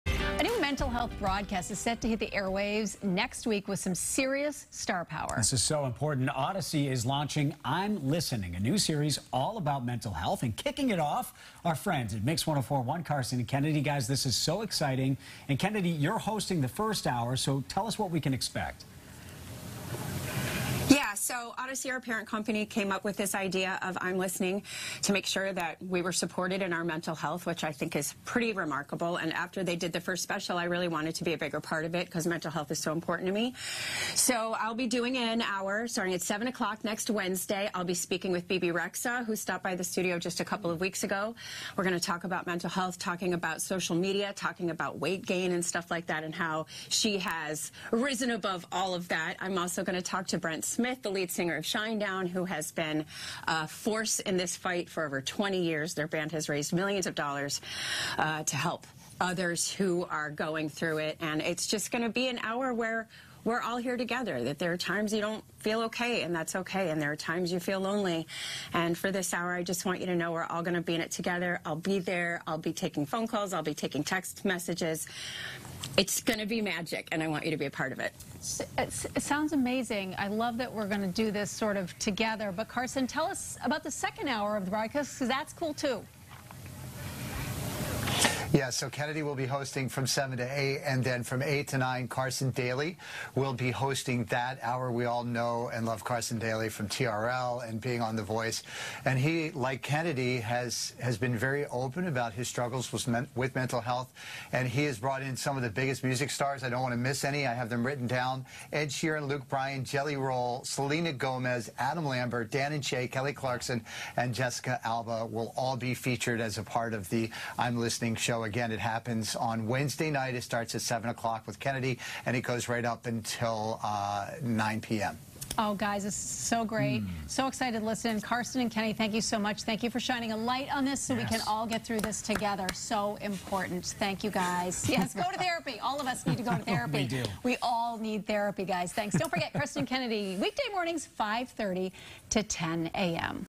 Audacy launching "I'm Listening" mental health broadcast September 20th
Mix 104.1's Karson & Kennedy joined the Morning Mix to chat about "I'm Listening".